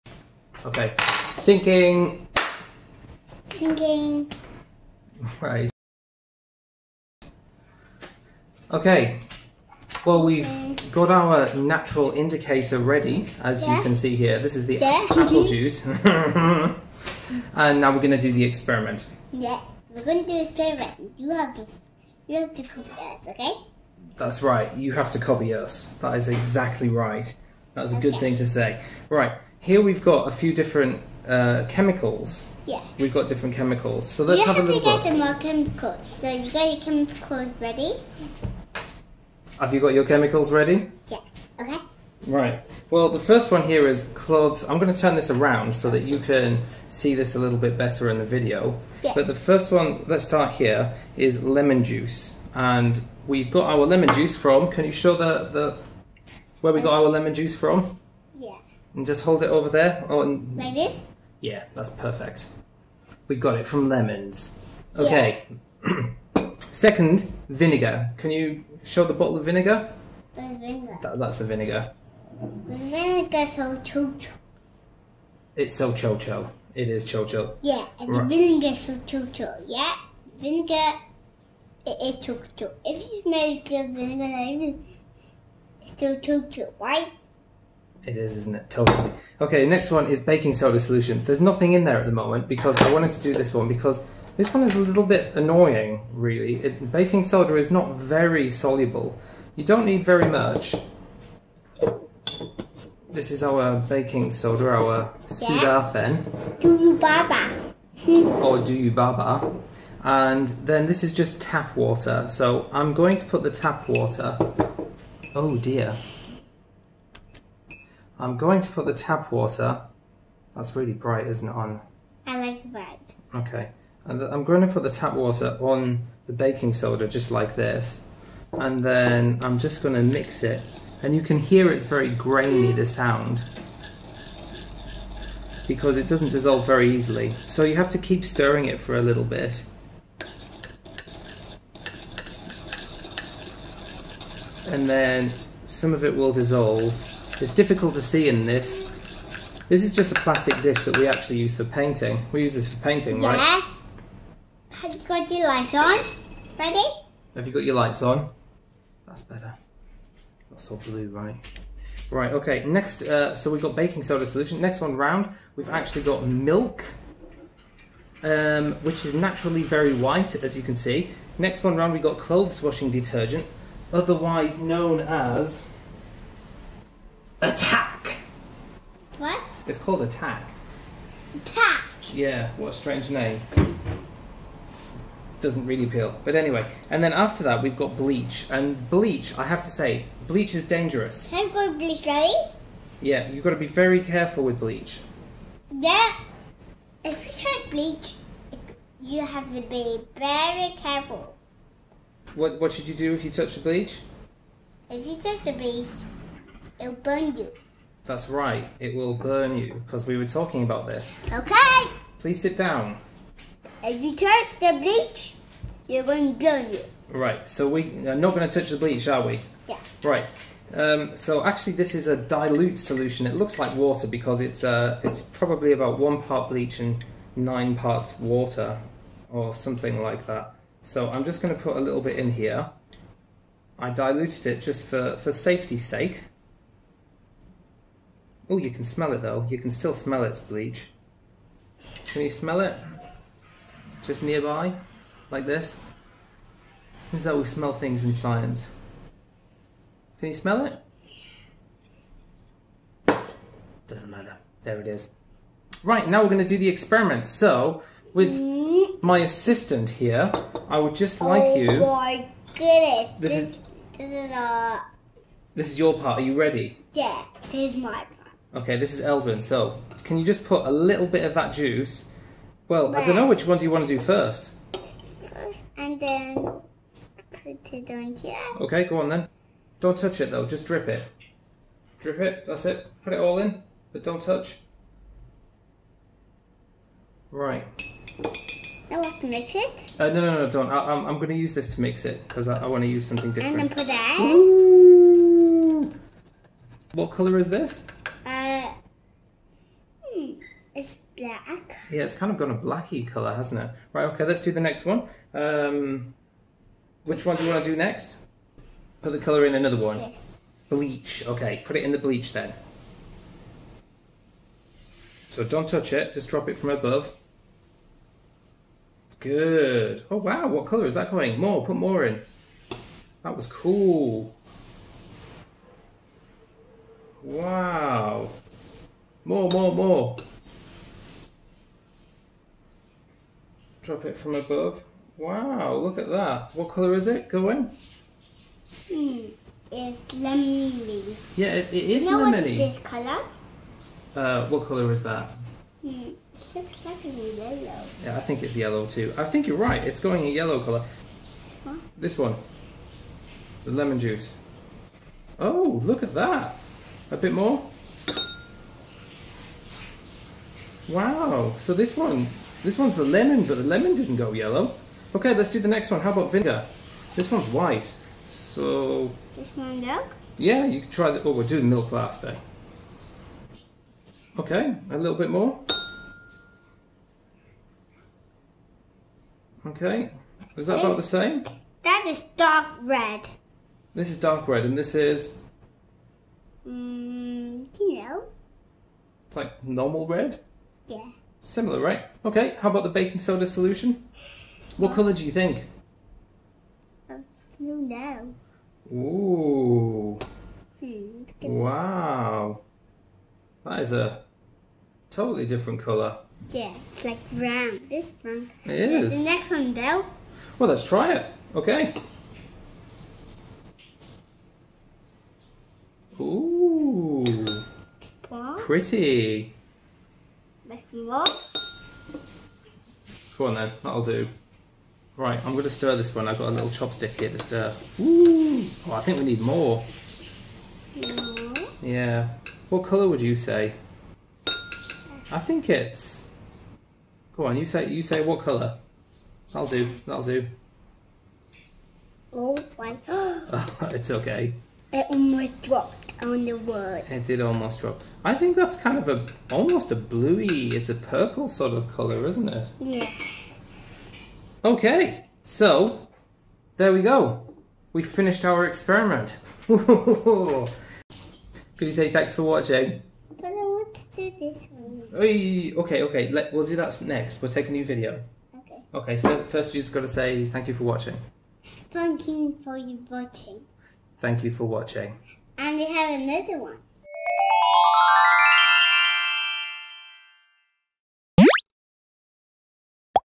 Natural pH Indicator - Red Apple Skin (part 2 of 3)
These videos will go through the following:
Part 1 - Planning - This is preparing the indicator solution
Part 2 - Experiment - This is using the indicator to test 6 substances
Part 3 - Discussion - This is processing and thinking about the results